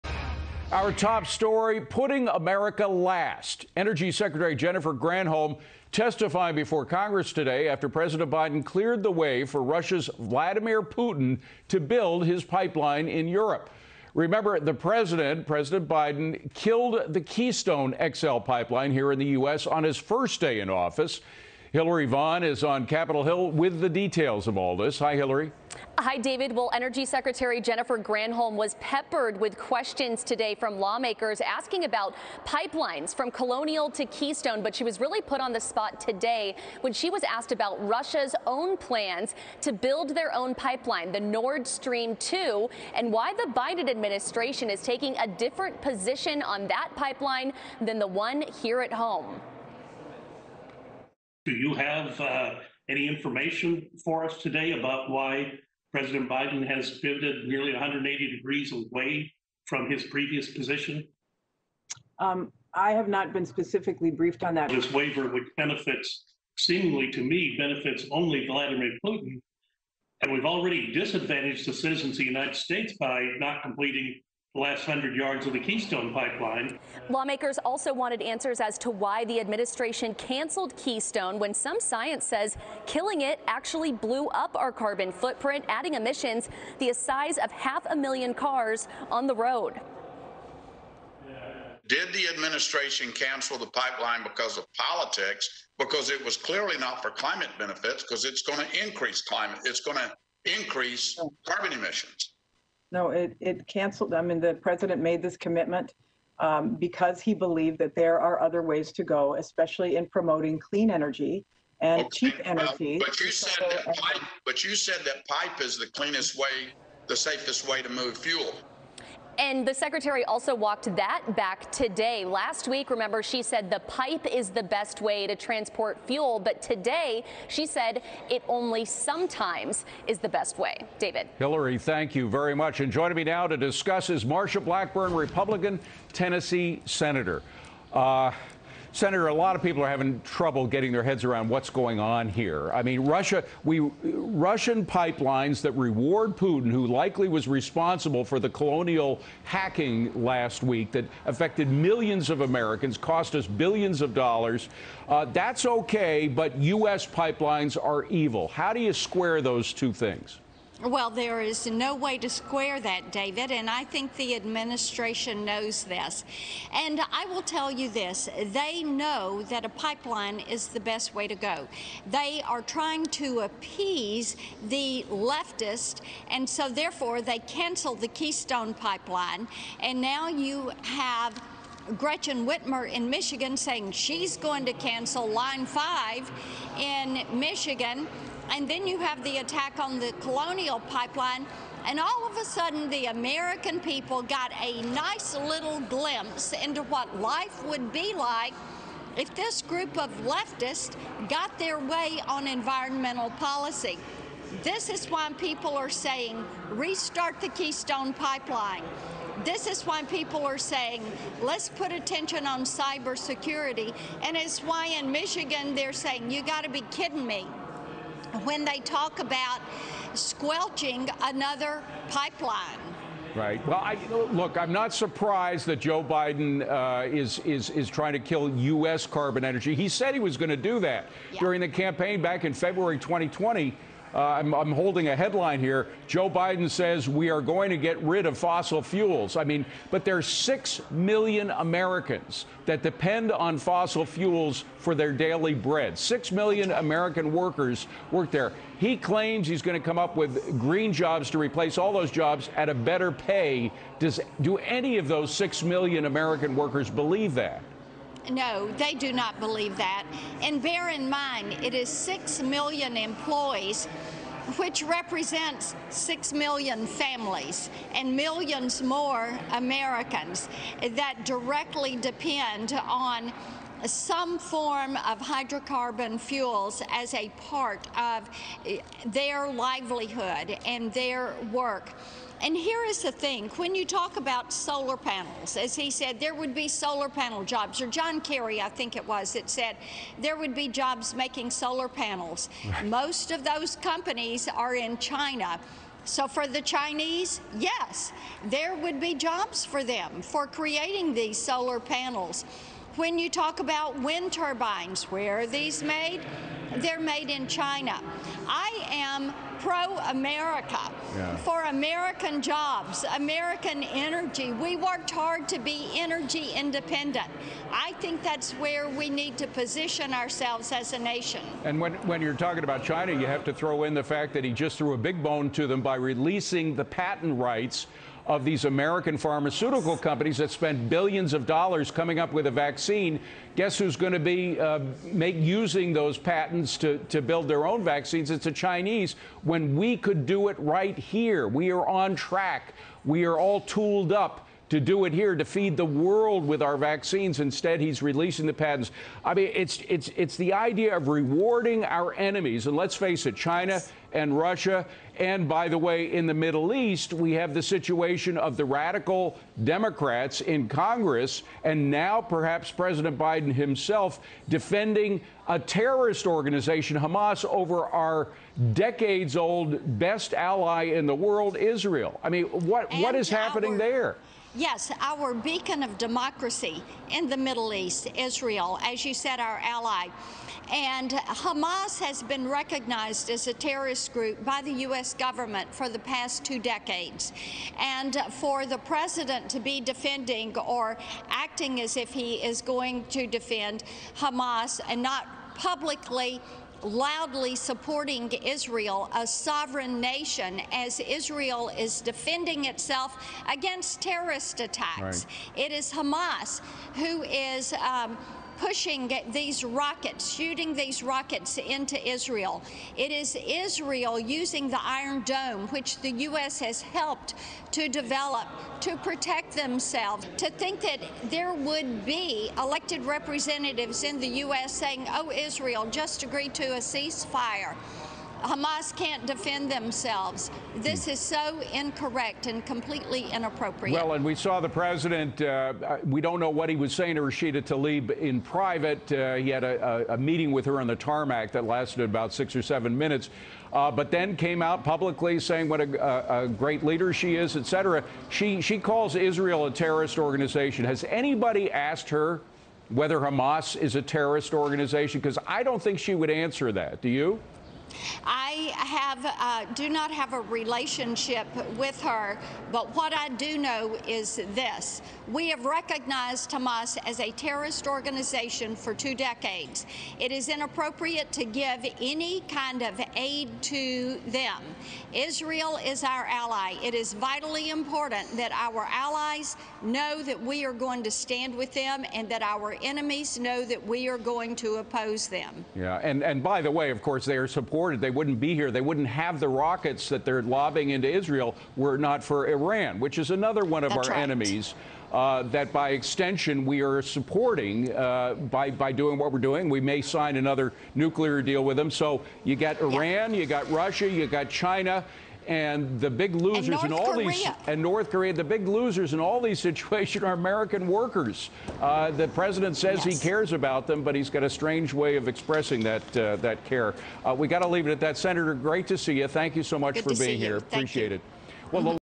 Energy Secretary pressed on Biden's decision to clear way for Russia pipeline
Sen. Marsha Blackburn, R-Tenn., reacts to the administration cancelling US pipelines while allowing Russia's.